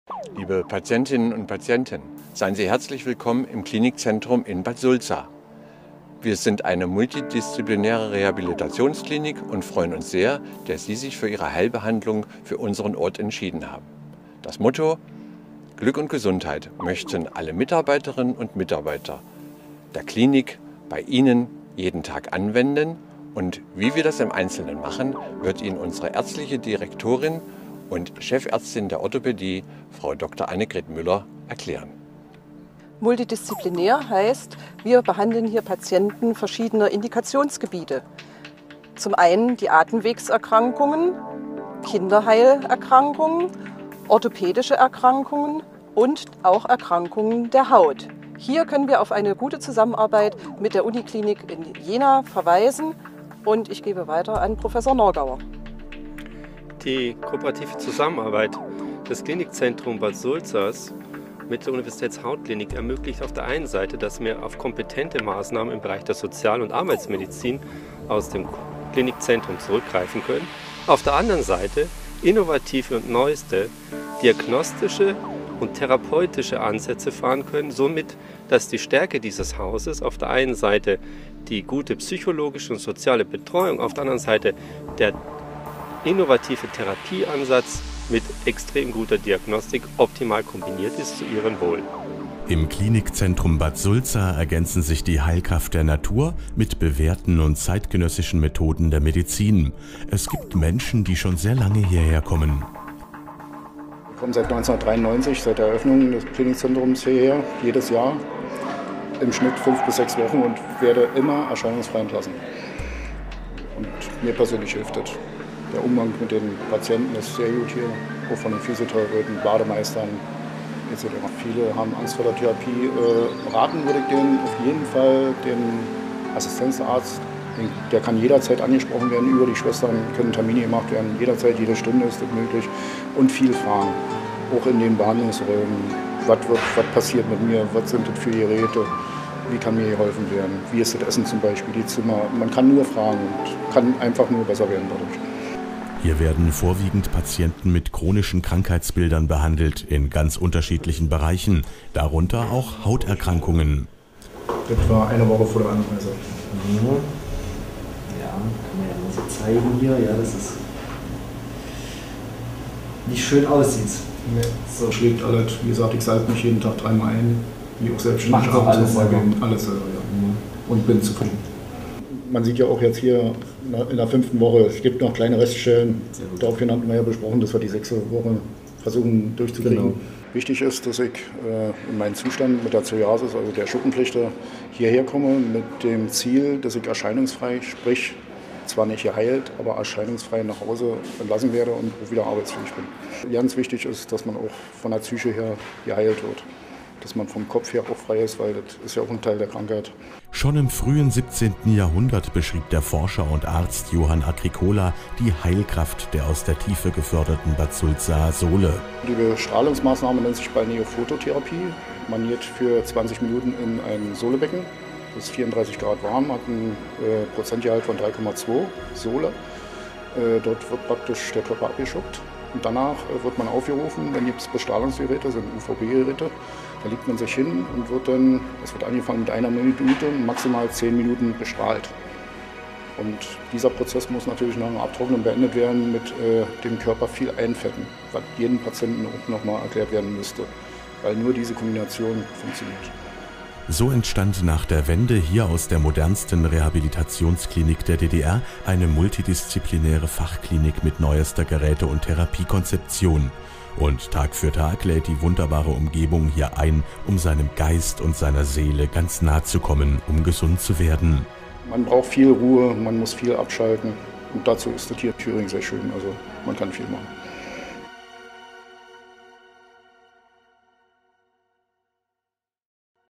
Im Klinikzentrum Bad Sulza in besten Händen
Im Klinikzentrum Bad Sulza ergänzen sich die Heilkraft der Natur und zeitgenössische Methoden der Medizin. Tag für Tag lädt das herrliche Thüringer Umfeld ein um sich zu erholen. Und so gibt es Kurgäste die schon seit vielen Jahren hier her kommen und sogar noch ein paar Tipps mitbringen.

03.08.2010